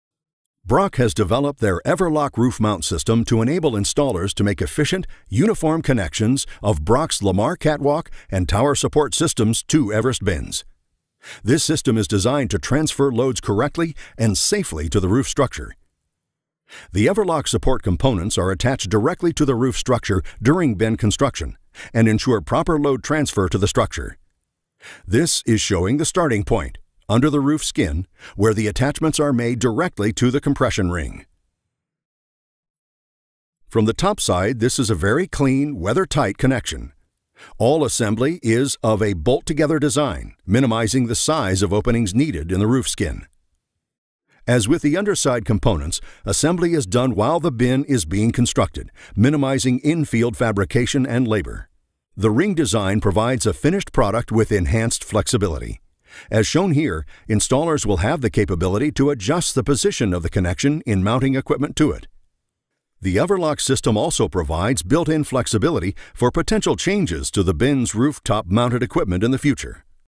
Brock Grain Storage - EverLoc Roof Mount System
Brock’s EVERLOC® Roof Mount System efficiently and uniformly
connects bins with Brock’s LeMar® Catwalk and Tower Support Systems. This helps control how the peak load is transferred to the bin’s compression ring.

With Higher Bin Roof Peak Loading, it is crucial to safely transfer loads correctly and directly into the roof structure. The Brock EVERLOC® Roof Mount System’s support components are attached directly to the roof structure during bin construction to ensure proper load transfers into the structure.

0:00 - Introduction
0:17 - Construction Overview